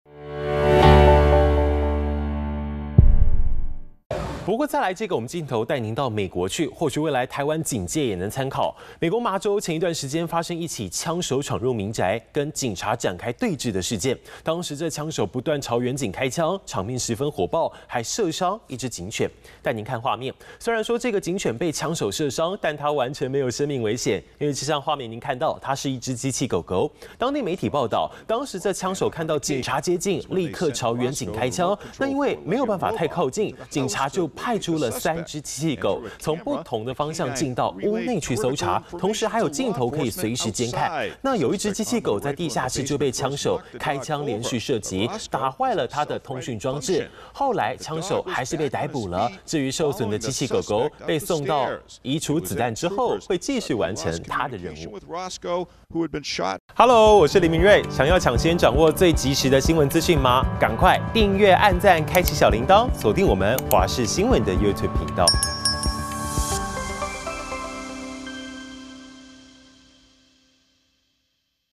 美民宅爆槍戰！ 機器戰犬護警立功 槍手遭逮｜華視新聞 20240329
美國麻州日前發生一起槍手闖入民宅，跟警察展開對峙的事件，當時槍手不斷朝警方開槍，場面十分火爆，還射傷了一隻警犬。